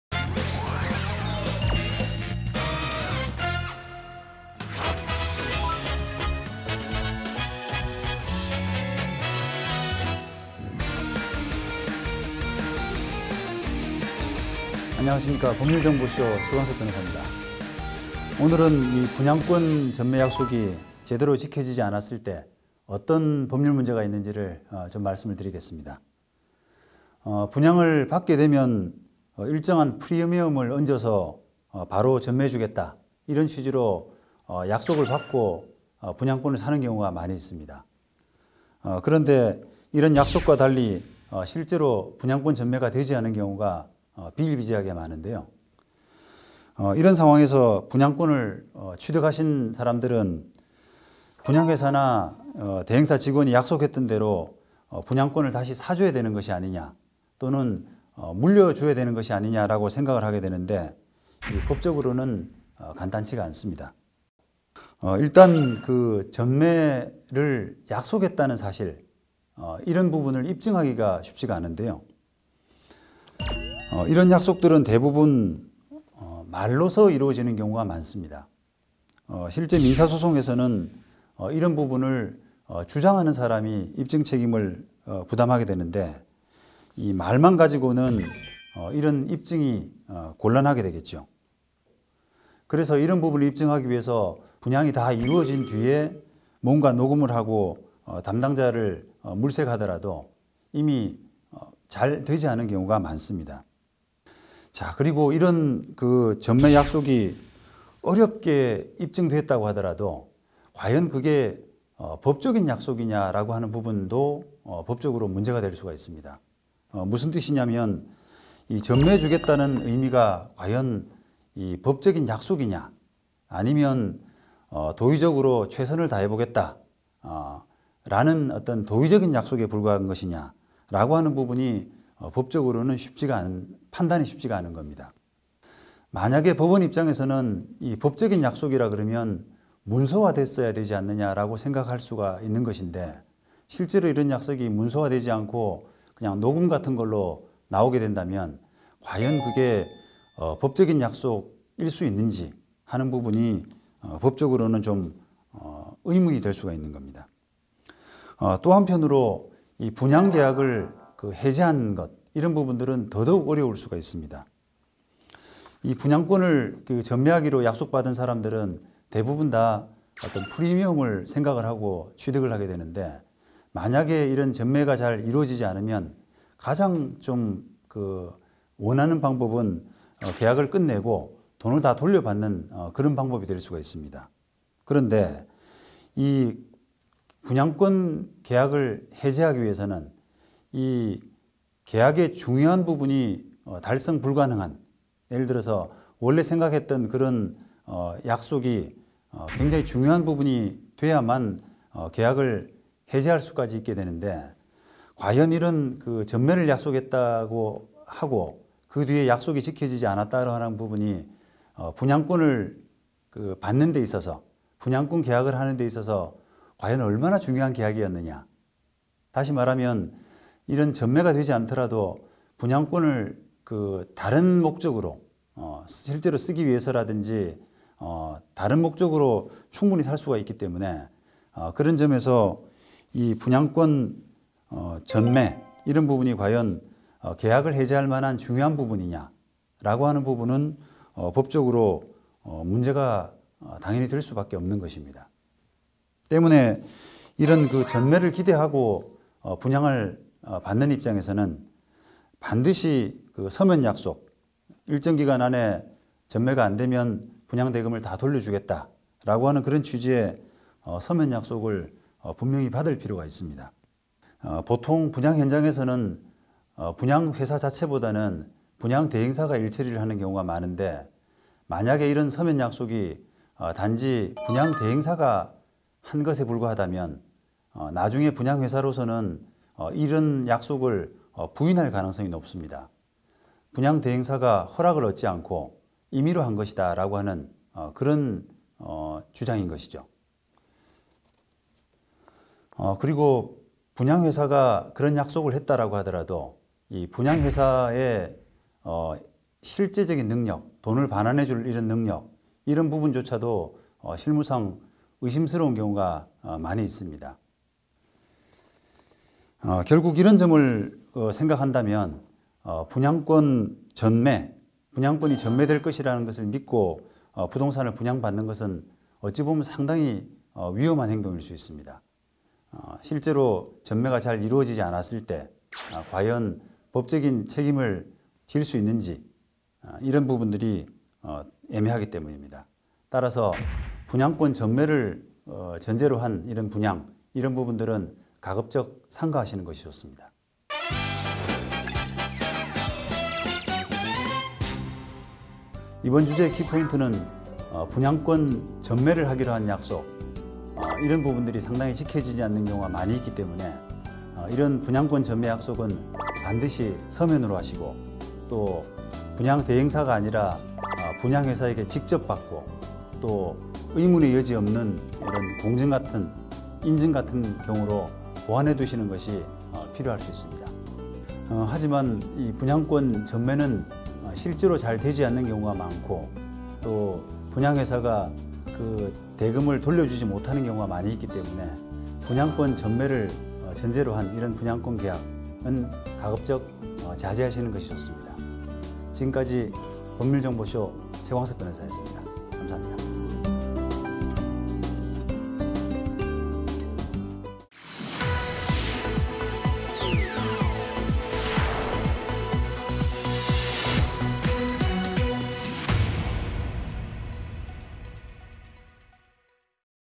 40th_법률정보 SHOW_ 최광석 변호사_분양권 전매 문제
분양권 전매 문제

각 분야 전문 변호사 5인방이 다양한 법률 분야에 대해 알기 쉽게

풀이해 드립니다.